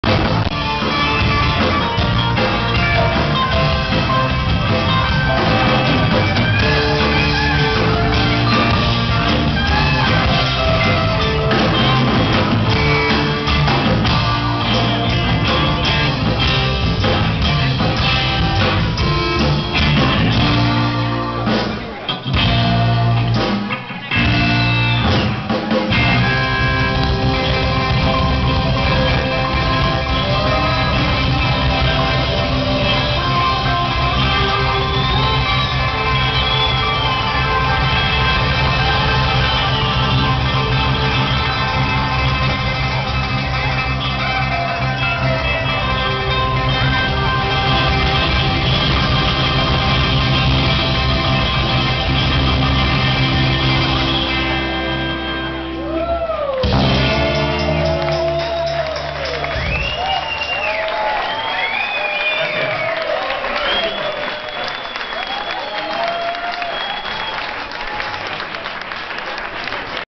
Angels Of Mercy - Festival Emergenza (Sala Mephisto)
Angels Of Mercy al festival Emergenza. Sala Mephisto 27/2/2010